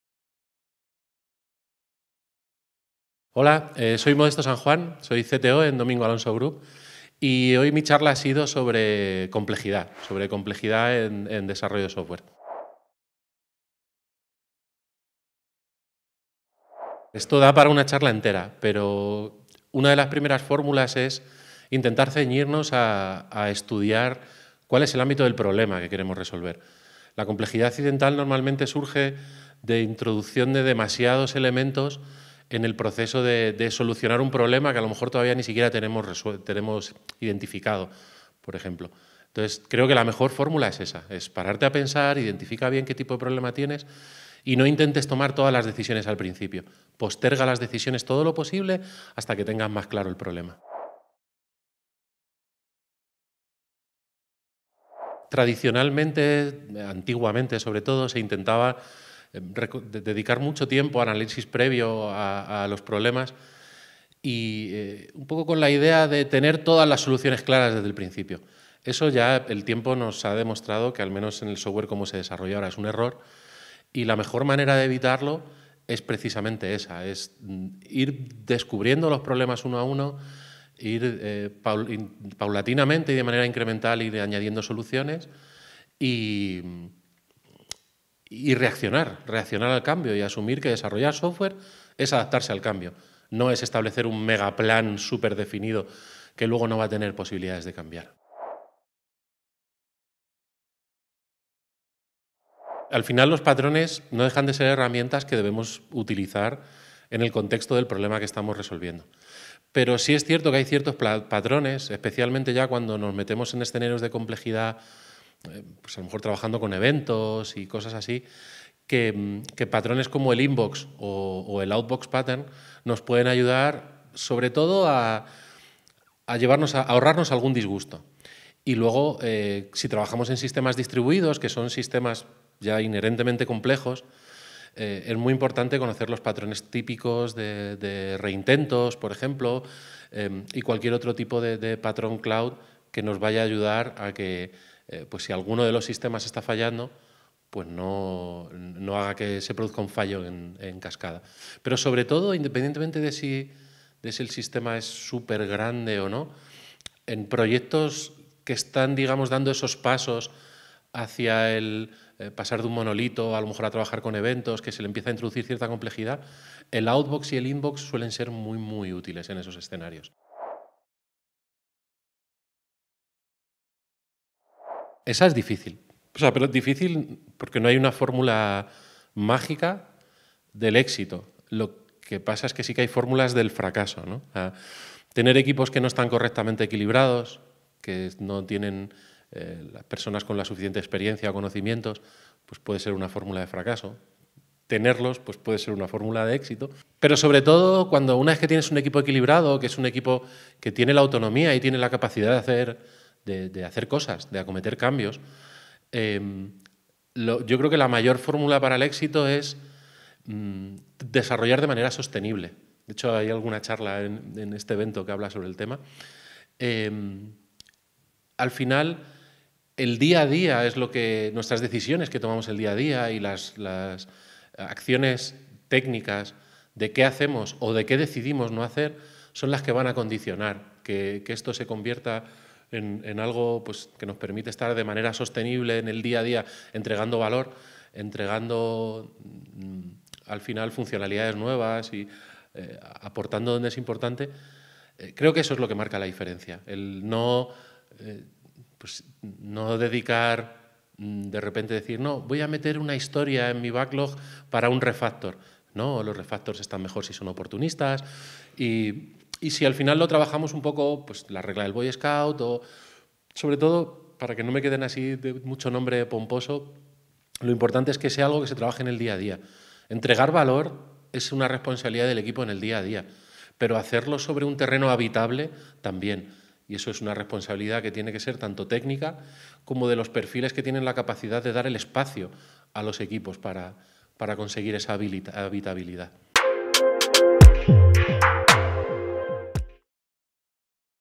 Entrevista a Modesto San Juan en Commit Conf 2023
Pregunta 1: ¿Cómo evitar la introducción de complejidad accidental en tus desarrollos?
Pregunta 2: ¿Cómo abordar la complejidad de manera efectiva en el desarrollo del software?
Pregunta 3: ¿Qué patrones de resiliencia recomiendas para evitar desarrollos complejos?
Pregunta 4: ¿Cómo lograr un buen equilibrio entre las necesidades del negocio y la calidad del código?